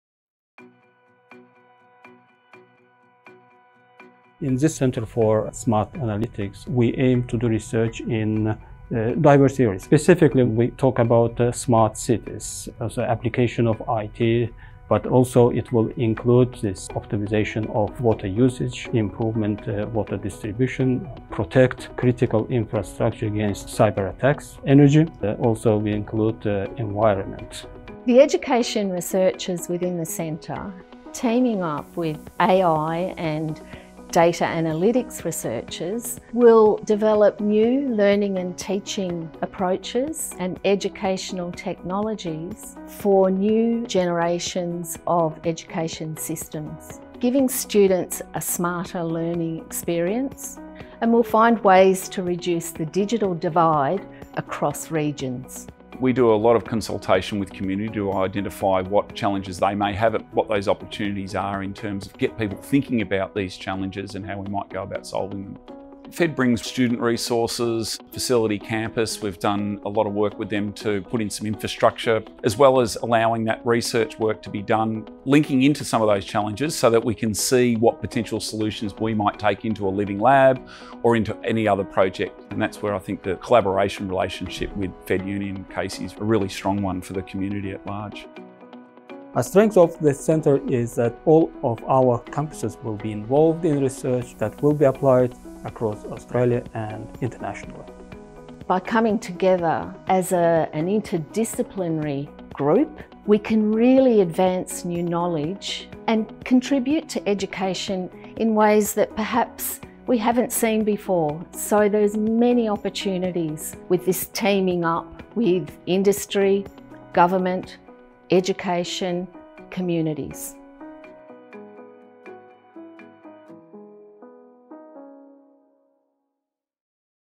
CSA - Centre for Smart Analytics
Established in 2022, the Centre for Smart Analytics (CSA) brings together expertise from across Federation University and its partner organisations.

These multidisciplinary teams are working to develop new knowledge and innovative solutions to the grand challenges of smart and resilient cities, regions and industries – solutions that positively impact lives and communities, both nationally and internationally.

CSA collaborates with regional communities, government and industry partners and researchers from national and overseas research organisations. In particular, it leverages its strong existing and emerging research collaboration opportunities with IBM and City Councils of Casey, Ballarat, Grampians and the Wimmera and Latrobe Valley. It works closely with the state and commonwealth governments and contributes to national capacity building through various initiatives.